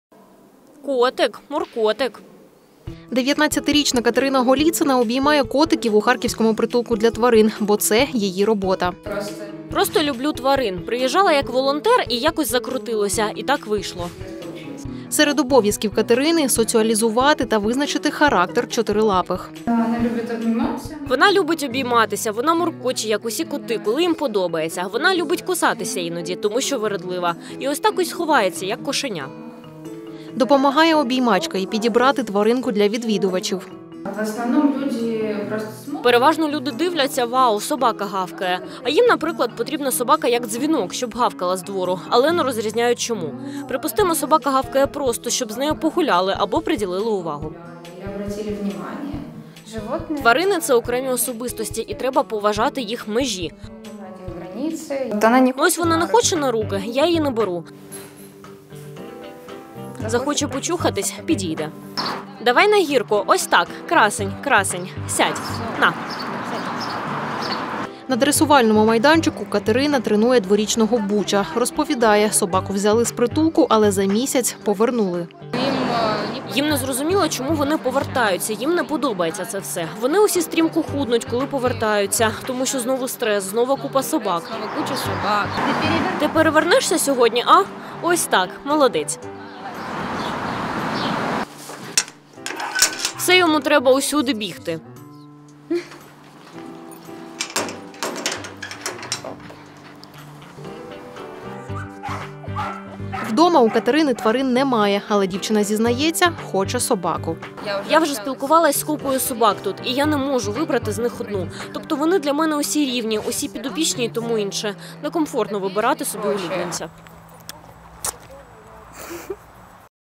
17.07.20 «Унікальні жіночі професії»: Обіймачка тварин
Пестити, обіймати та вигулювати чотирилапих. Такі посадові обов'язки обіймачки тварин з харківського притулку Катерини Галіциної.

Як проходить робочий день дівчини та скільки разів вона обіймає песиків та котиків — у рубриці «Унікальні жіночі професії».